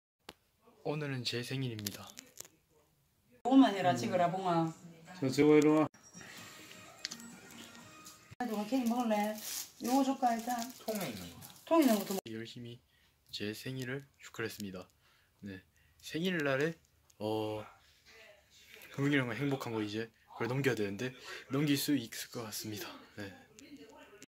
나의 생일
나의20살(만19세)생일이었다.(2021.2.4)
본영상은2021년2월4일 영상입니다